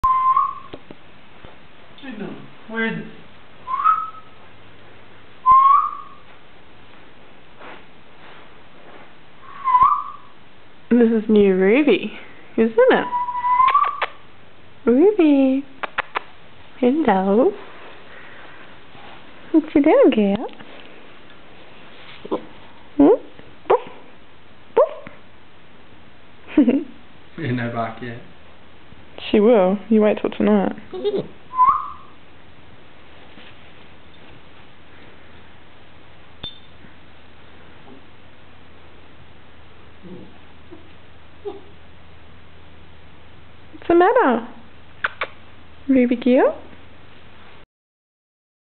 weimaraner puppy 8 weeks
ruby's first day home with her new mum and dad